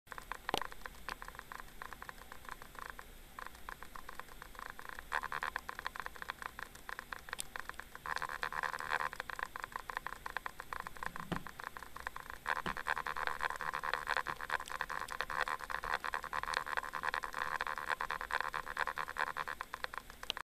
fake blood effect film
I'm practicing making little scenes before i make an official film! In this i tried my own idea for making fake blood, and there's a lighting affect. i look dead too!